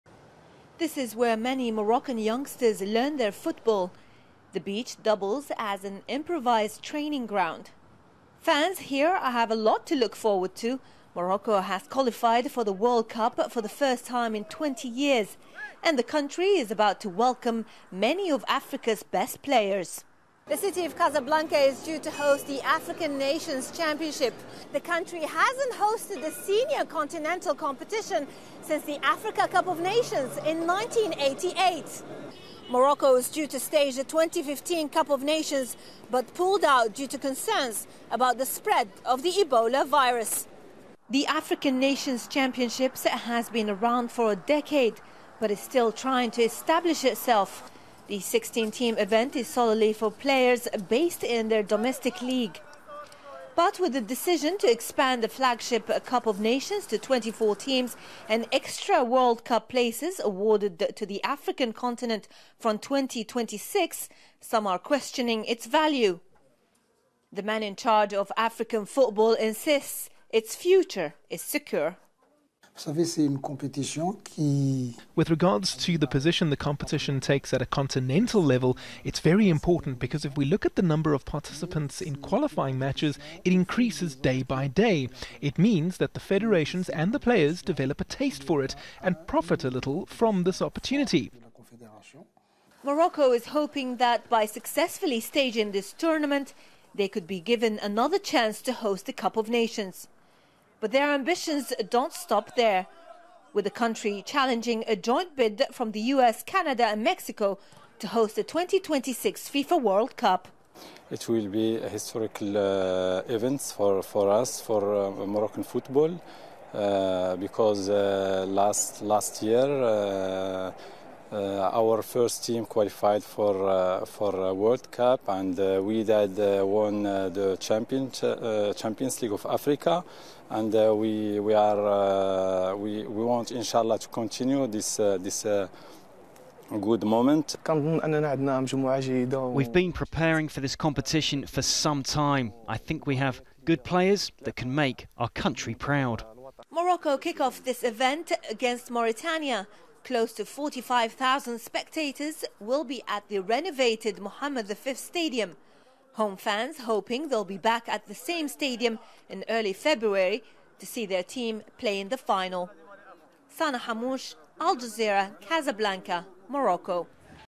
Morocco 🇲🇦 ready to host African Nations Championship
In the next few hours Morocco will kick off against Mauritania to get the fifth edition of the African Nations Championship underway in Casablanca.

It's the biggest tournament on the continent for home-grown talent.

Morocco's football fortunes are on the rise after qualifying for this year's World Cup.

Close to 45,000 spectators will be at the renovated Stade Mohammed V.

The country is hoping that this tournament will be a stepping stone to bigger things in the future.

Al Jazeera's Sana Hamouche reports from Casablanca, Morocco.